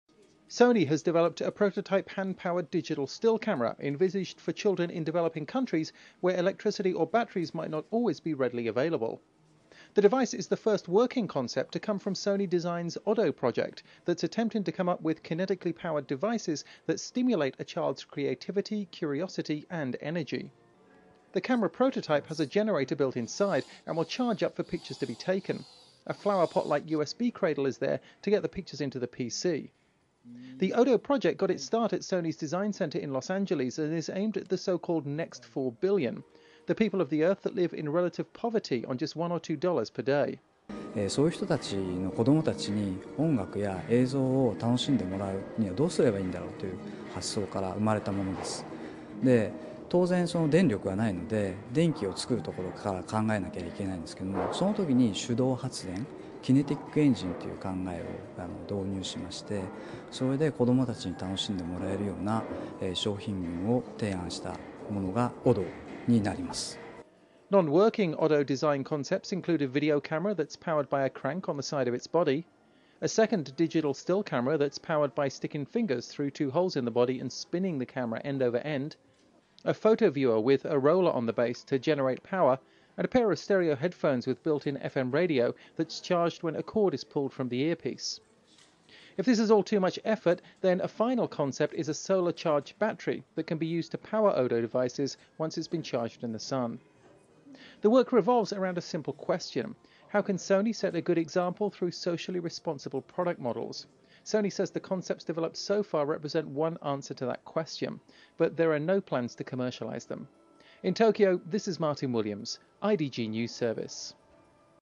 Sony shows hand-powered digital camera
Sony has developed a working prototype hand-powered digital still camera. The camera is the first working concept in Sony's Odo design program that seeks ideas for products for use in areas where the electricity supply is weak or non-existant.